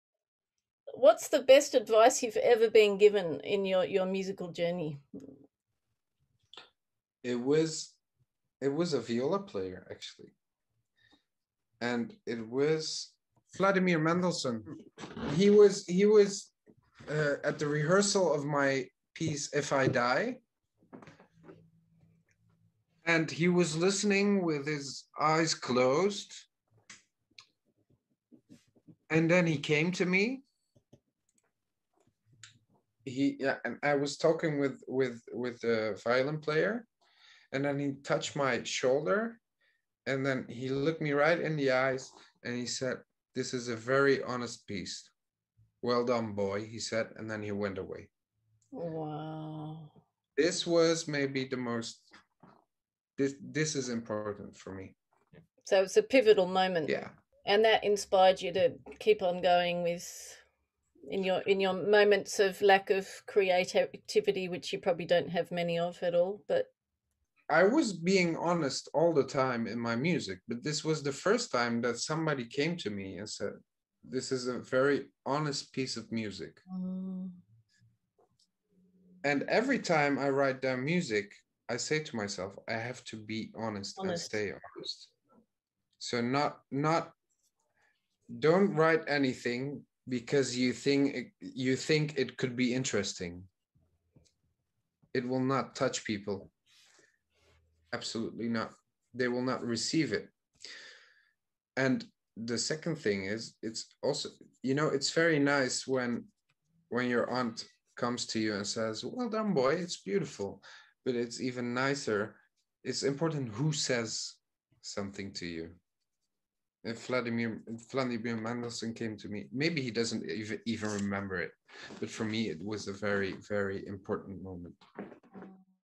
IT WAS VLADIMIR MENDELSSOHN!
Just read that violist Vladimir Mendelssohn past away on the 13th of August. A few months ago during an interview I talked about a pivotal moment that I had with him. Condolences to his family and relatives.